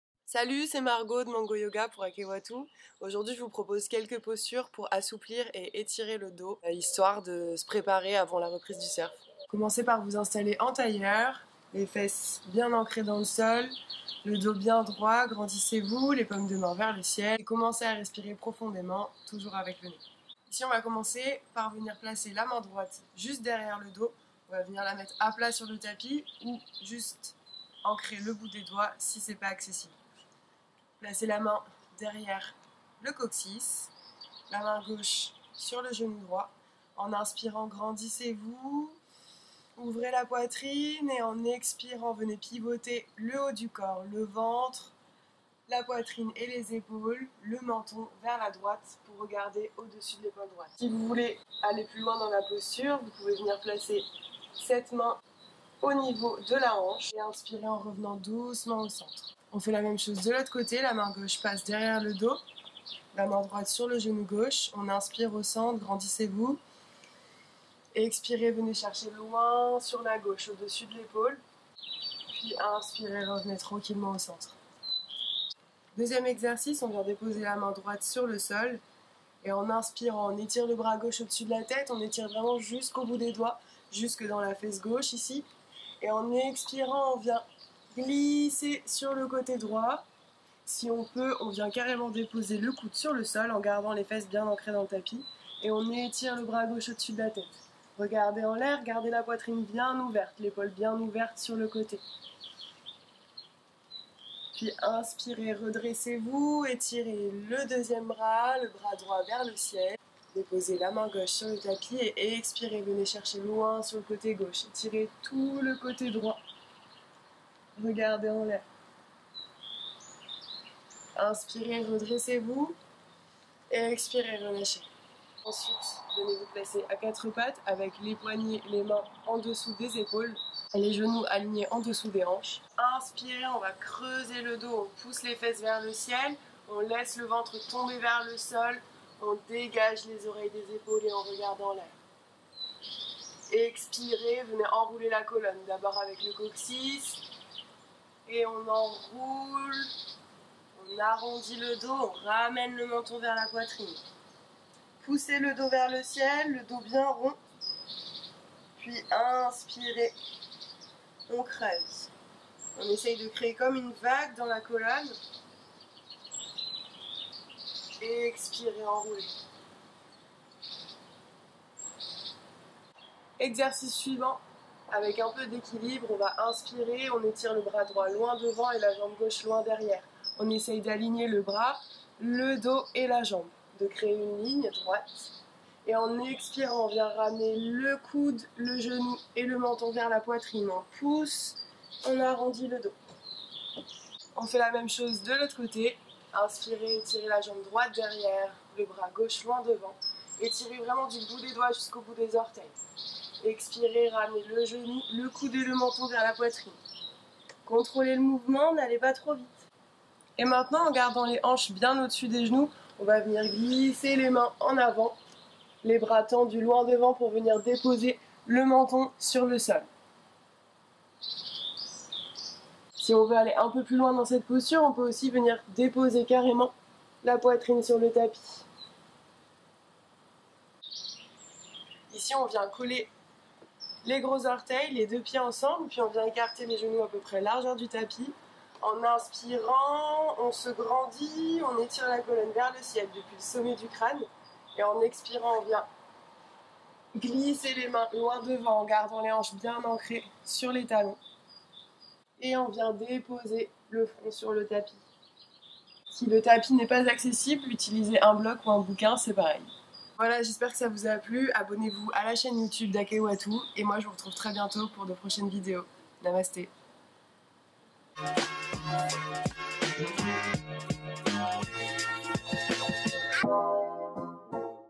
YOGA - ENTRAÎNEMENT AU SURF (ÉPISODE 1)
Mango Yoga te donne les secrets pour te préparer au surf depuis chez toi !